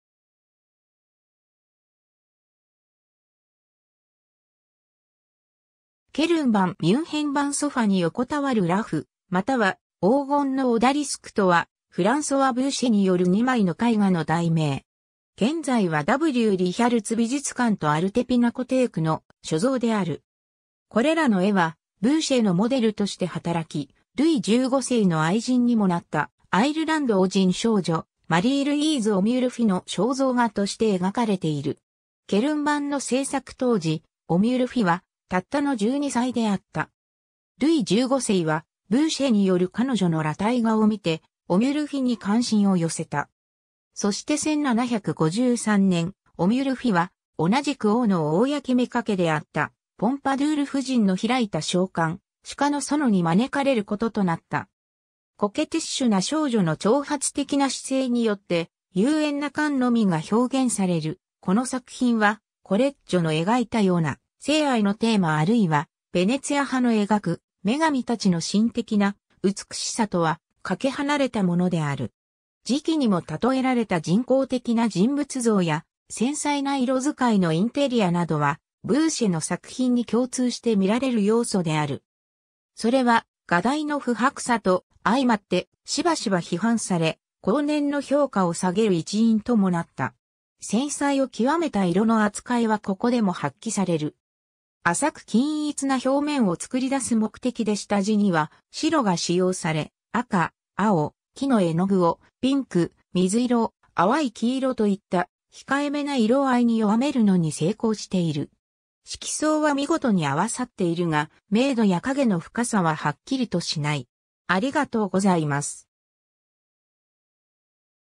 ソファに横たわる裸婦
アルテ・ピナコテークの所蔵作品
ヴァルラフ・リヒャルツ美術館の所蔵作品
フランソワ・ブーシェの作品
ケルン版 ミュンヘン版 ソファに横たわる裸婦または黄金のオダリスク（フランス語: Jeune fille allongée、Jeune fille couchée、L'Odalisque blondeとも）とは、フランソワ・ブーシェによる2枚の絵画の題名。
 現在はW・リヒャルツ美術館とアルテ・ピナコテークの所蔵である。
 これらの絵は、ブーシェのモデルとして働き、ルイ15世の愛人にもなったアイルランド人少女、マリー＝ルイーズ・オミュルフィの肖像画として描かれている。
ケルン版の制作当時、オミュルフィはたったの十二歳であった。
 ルイ15世は、ブーシェによる彼女の裸体画を見てオミュルフィに関心を寄せた。
そして1753年、オミュルフィは同じく王の公妾であったポンパドゥール夫人の開いた娼館、鹿の園に招かれることとなった。
 コケティッシュな少女の挑発的な姿勢によって、優艶な官能美が表現されるこの作品は、コレッジョの描いたような❝聖愛❞のテーマあるいは、ヴェネツィア派の描く女神たちの神的な美しさとはかけ離れたものである。
 磁器にも喩えられた人工的な人物像や、繊細な色遣いのインテリアなどはブーシェの作品に共通して見られる要素である。
それは画題の浮薄さと相俟ってしばしば批判され、後年の評価を下げる一因ともなった。
繊細を極めた色の扱いはここでも発揮される。
浅く均一な表面を作り出す目的で下地には白が使用され、赤、青、黄の絵の具を、ピンク、水色、淡い黄色といった控えめな色合いに弱めるのに成功している。
色相は見事に合わさっているが、明度や影の深さははっきりとしない。